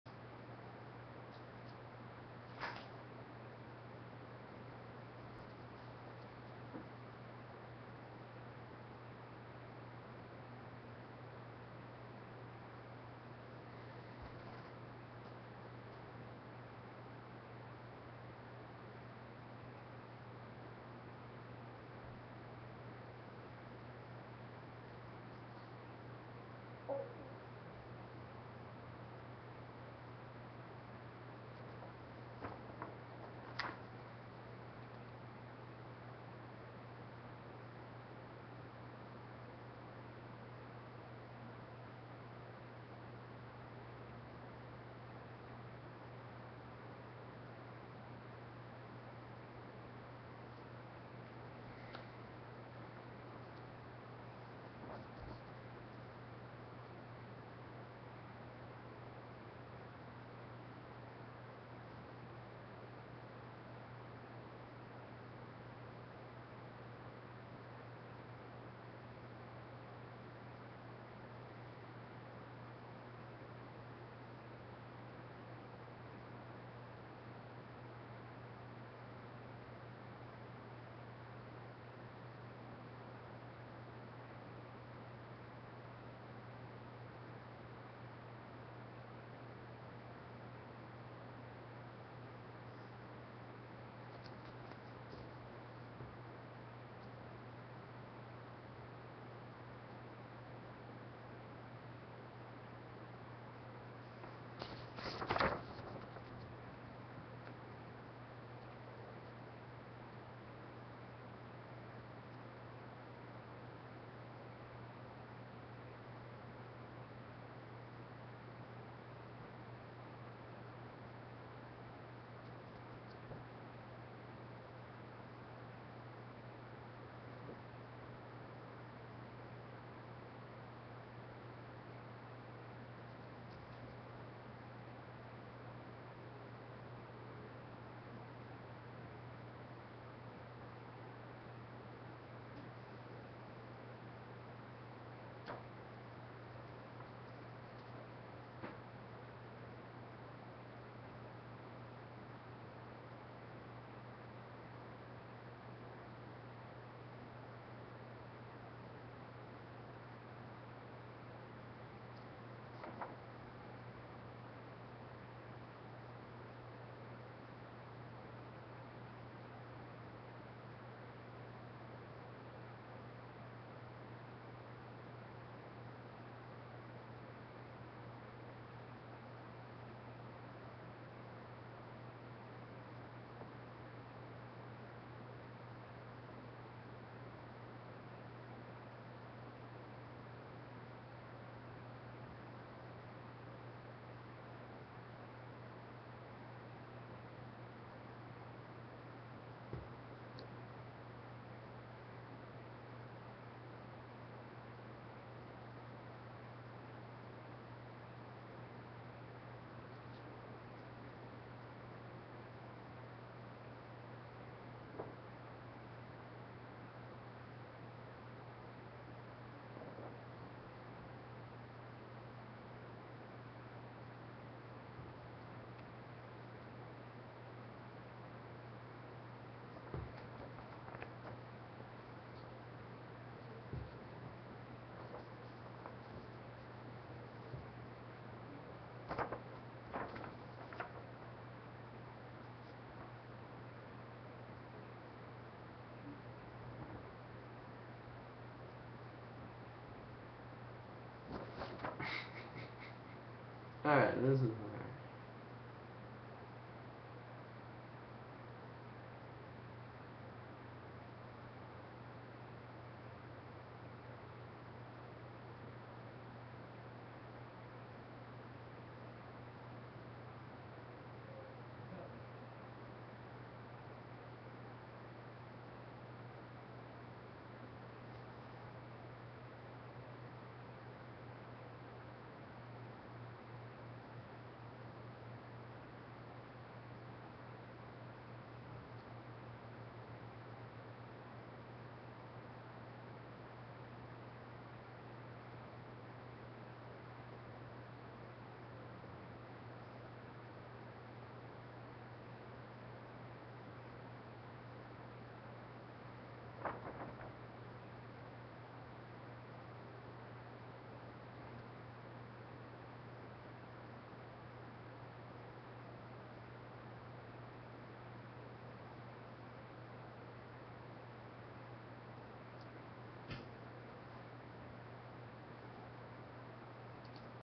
the door frame 3 rectangles no bottom rectangle artwork pattern design based on frames.
this art is about a door frame pattern . not drugs its about door frame patterns color design youtube just picked the wrong video tag for my art im a kind peaceful polite guy who likes drawing colorfull things im sober as puck puck its amazeing how so many people get the wrong art message im trying to show you she's what i really want. im autistic art designer of mystery.